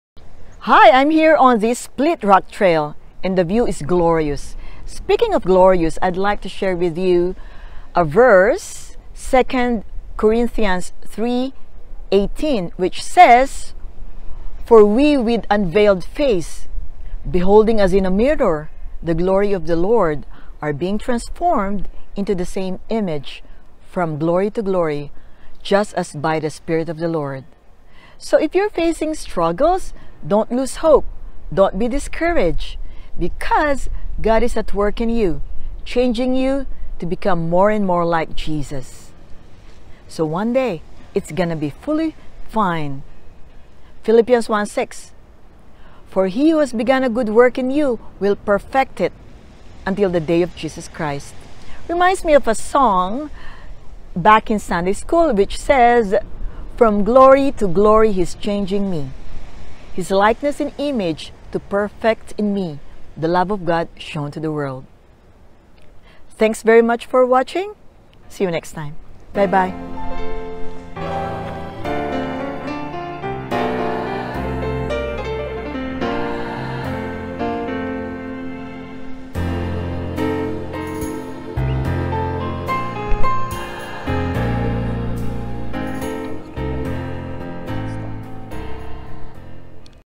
God is changing us from glory to glory. 2 Corinthians 3:18
But we all, with unveiled face, beholding as in a mirror the glory of the Lord, are being transformed into the same image from glory to glory, just as by the Spirit of the Lord. 2 Corinthians 3:18
Child of God, if you're tired of your struggles, don't lose hope, because God is changing you each day to be more and more like Jesus. One day, it will be fully fine, for "He who began a good work in you will perfect it until the day of Christ Jesus (Phil. 1:6)." Nothing can separate you from the love of God.